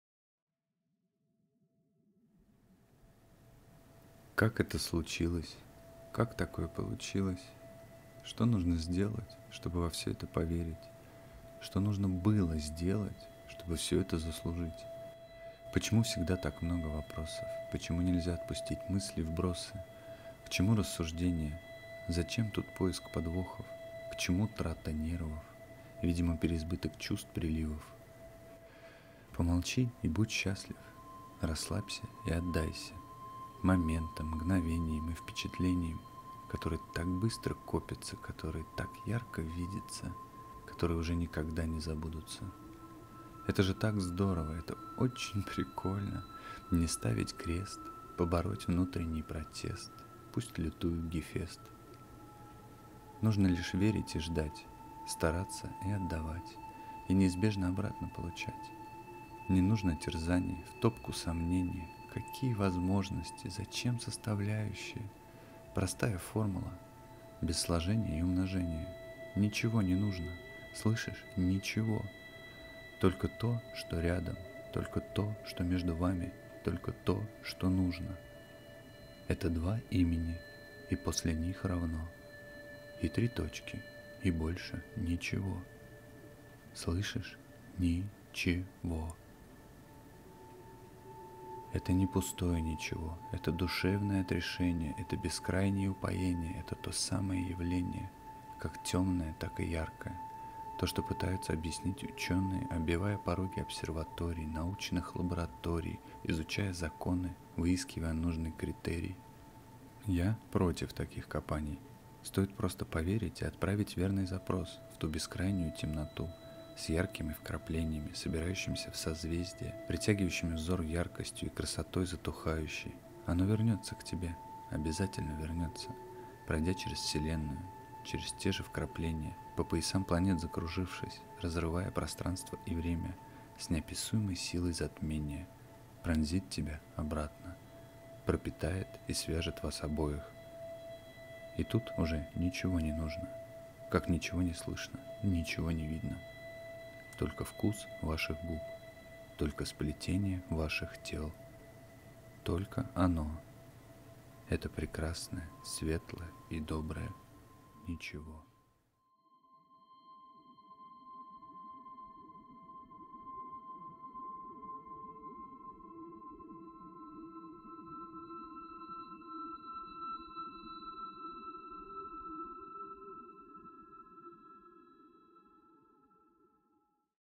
вкрапления.. | Читает автор: Денис Барановский
как это случилось
как такое получилось
что нужно сделать
чтобы во всё это поверить
что нужно БЫЛО сделать
чтобы всё это заслужить
почему всегда так много вопросов
почему нельзя отпустить мыслей вбросы
к чему рассуждения
зачем тут поиск подвохов
к чему трата нервов
видимо переизбыток чувств приливов.

помолчи и будь счастлив
расслабься и отдайся
моментам, мгновениям и впечатлениям
которые так быстро копятся
которые так ярко видятся
которые уже никогда не забудутся
это же так здорово
это очень прикольно
не ставить крест
побороть внутренний протест
пусть лютует Гефест

нужно лишь верить и ждать
стараться и отдавать
и неизбежно обратно получать
не нужно терзаний
в топку сомнения
какие возможности?
зачем составляющие?
простая формула
без сложения и умножения
ничего не нужно
слышишь? ничего!
только то, что рядом
только то, что между вами
только то, что нужно
это два имени, и после них равно
и три точки и больше ничего!
слышишь? НИиЧЕГО!

это не пустое ничего
это душевное отрешение
это бескрайнее упоение
это то самое явление
как темное так и яркое
то что пытаются объяснить учёные
оббивая пороги обсерваторий
научных лабораторий
изучая законы,
выискивая нужный критерий

я против таких копаний
стоит просто поверить и отправить верный запрос
в ту бескрайнюю темноту
с яркими вкраплениями, собирающимися в созвездия
притягивающими взор яркостью и красотой затухающей
оно вернётся к тебе, обязательно вернётся
пройдя через вселенную, через те же вкрапления
по поясам планет закружившись
разрывая пространство и время
с неописуемой силой затмения
пронзит тебя обратно
пропитает и свяжет вас обоих
и тут уже ничего не нужно
как ничего не слышно, ничего не видно,
только вкус ваших губ, только сплетения ваших тел
только ОНО - это прекрасное светлое и доброе НИЧЕГО.